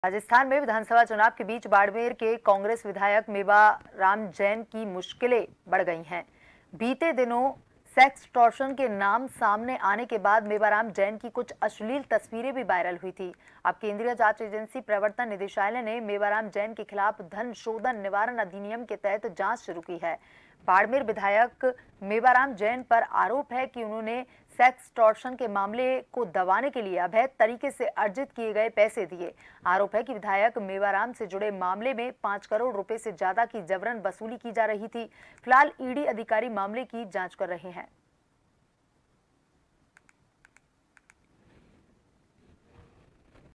Barmer कांग्रेस विधायक Mewaram Jain की बढ़ी मुश्किलें, ED ने शुरू की जांच
Watch JAN TV on :
Tata Play DTH : 1185
Airtel DTH: 355
JIO Fiber: 1384

NEWS CREDIT-GS